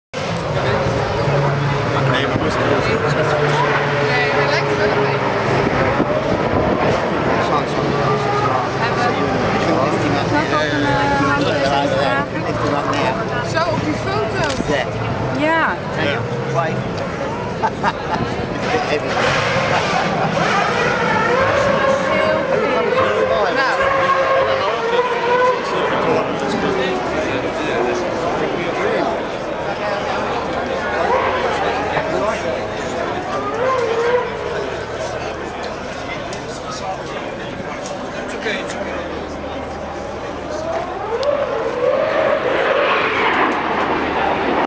Jochen Mass and Marco Werner signing autographs at Goodwood 2009
Please take a look for more candid photographs, hunting reports and authentic autographed photos